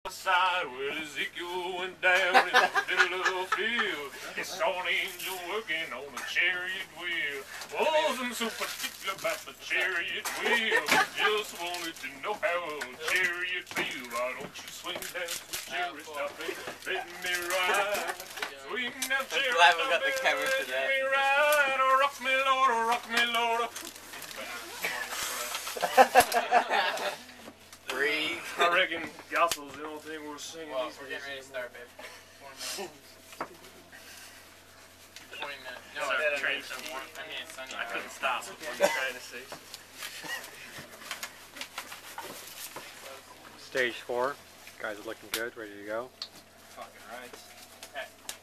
DZNUTS Tour of California
David applying DZ Nuts to his undercarriage at the Tour of California